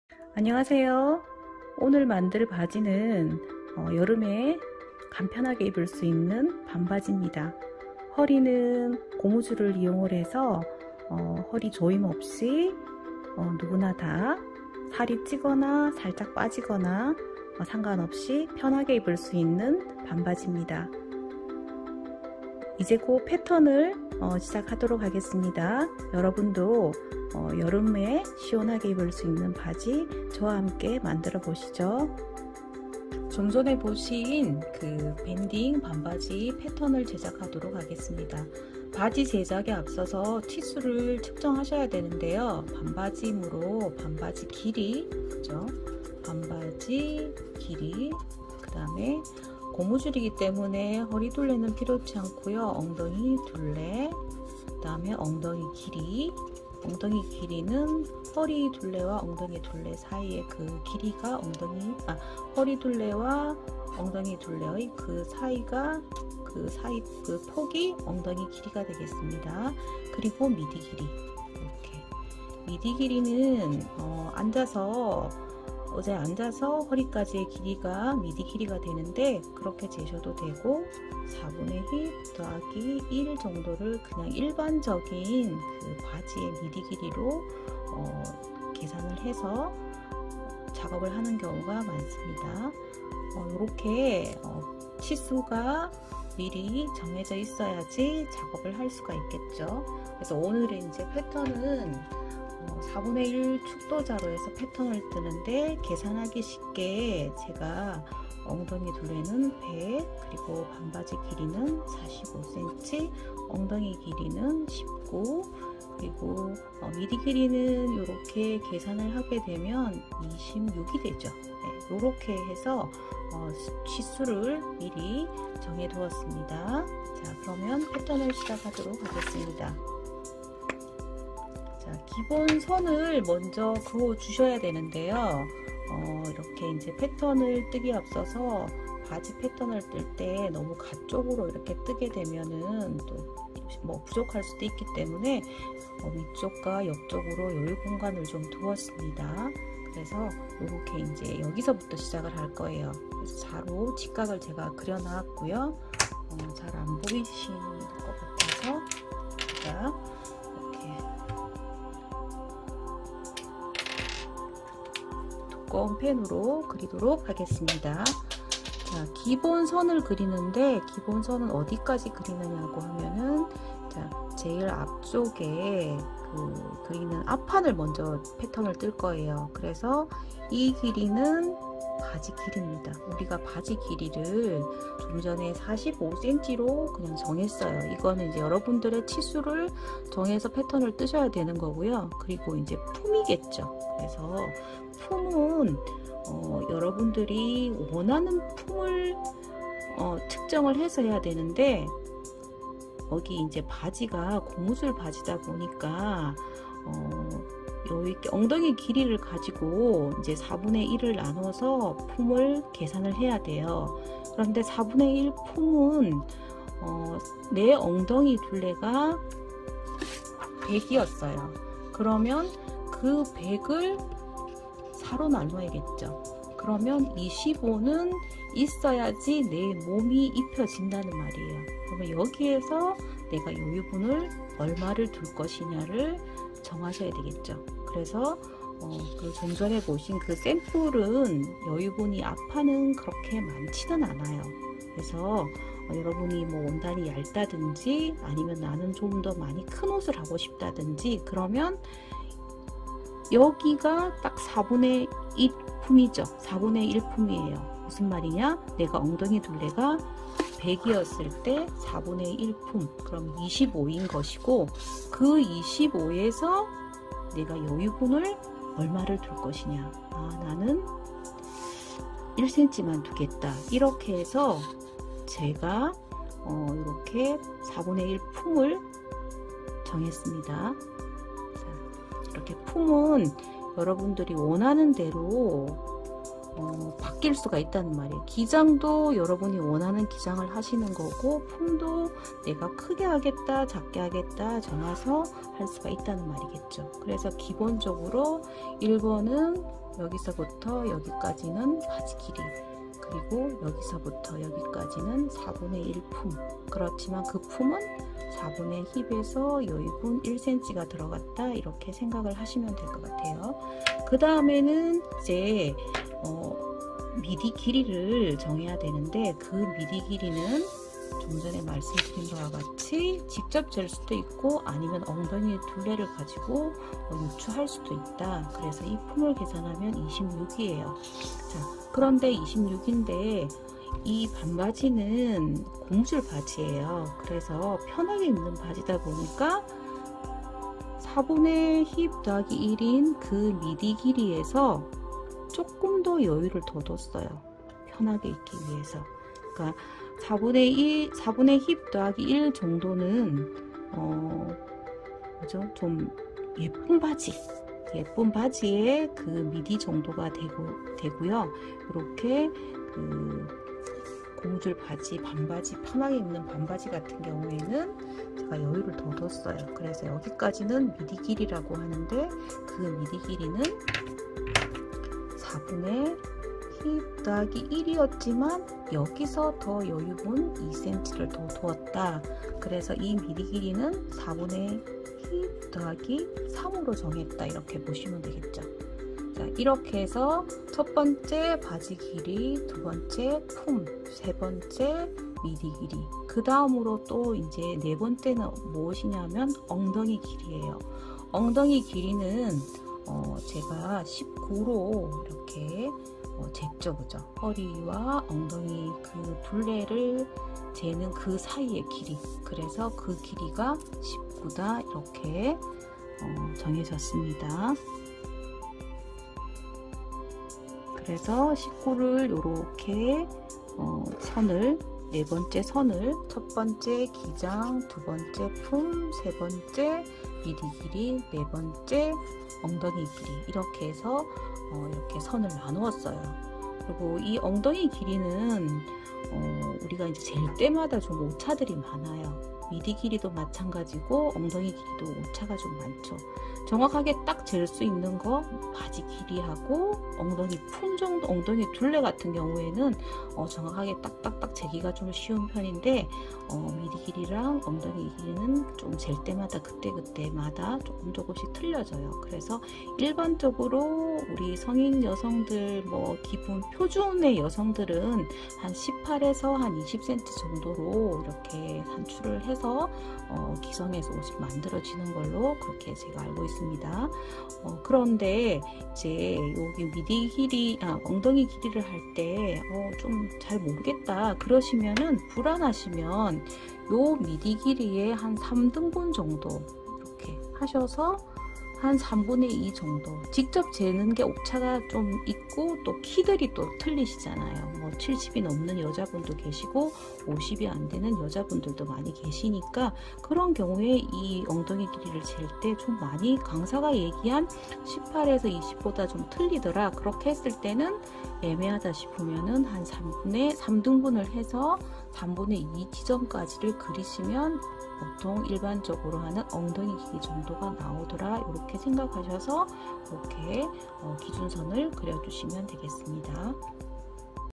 (eng) 하루만에 린넨반바지, 허리고무바지, 밴딩반바지 만들기 1편 (기초이해편)
- 재봉틀 사용 가능자부터, 쉽게 이해할 수 있는 반바지 패턴제작을 담은 영상입니다.
- 통바지, 기본바지까지 이 패턴 하나로 충분히 응용가능하도록 제작된 영상입니다.`
- A라인의 허리고무밴딩 반바지 패턴 제1편